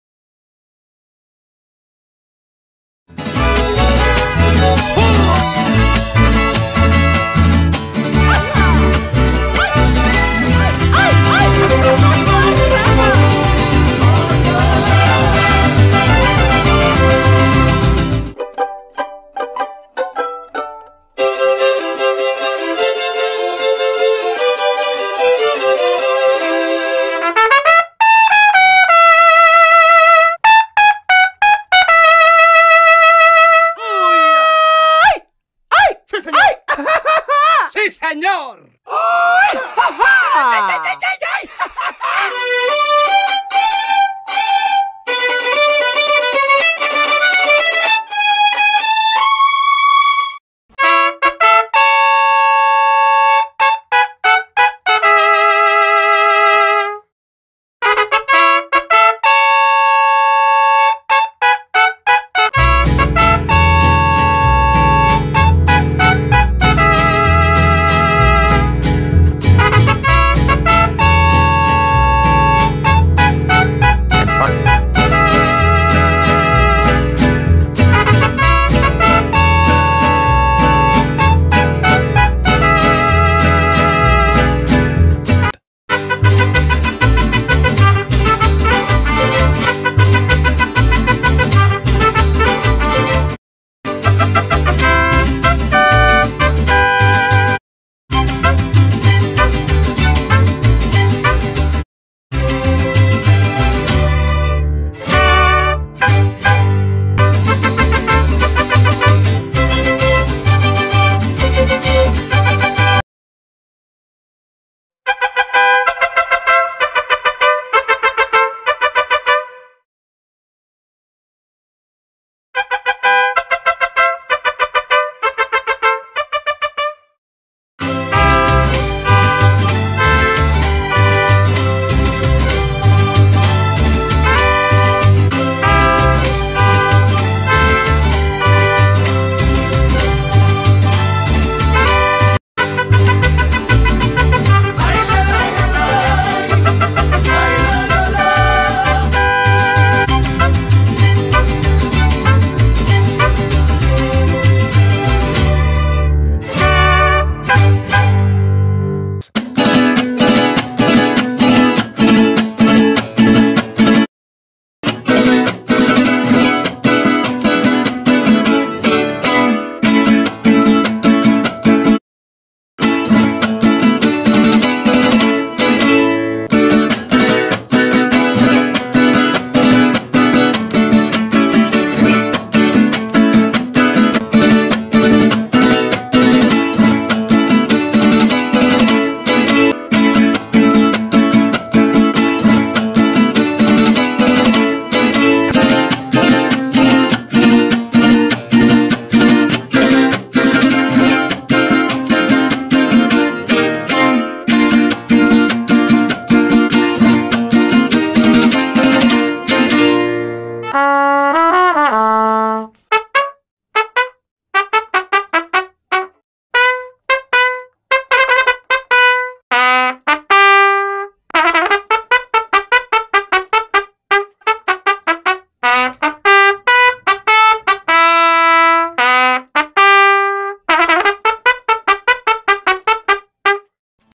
Samples kontakt MARIACHI MUSIC 2022
MARIACHI MUSIC se reproduce en vivo
cada bucle esta hecho a mano y agrega la cantidad justa de picante para ese ambiente autentico
que se anhela. los bucles violín 1st,2nd,3rd y trompeta 1st y 2nd se han separado
lo que brinda la opción única y la flexibilidad de crear armonías simples o complejas. tanto violín como la trompeta
contienen dos fases melódicas separadas para cada sección de canción de cada kit